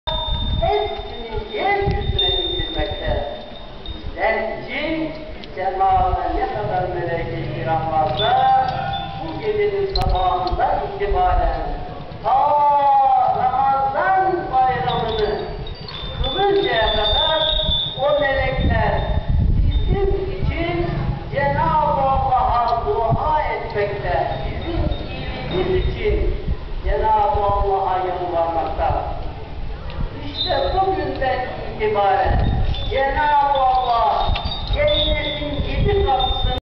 Ayvalık'ta Ayin:)
Ayvalık'ta bir Cuma günü... Cuma namazı için herkes camii de. Arka planda ezan sesi. Ilginc bir goruntu idi. Megerse, bir suredir bu halde imis videodaki sahis. Ayvalik'ta taninan biriymis. Herhangi bir alay konusu yoktur. Bilginize...